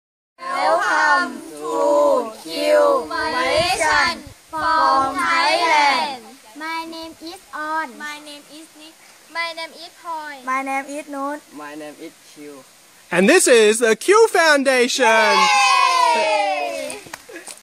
Q KIDS FEB 2015
Some of the kids from my charity that brings health, hope and happiness to children in need.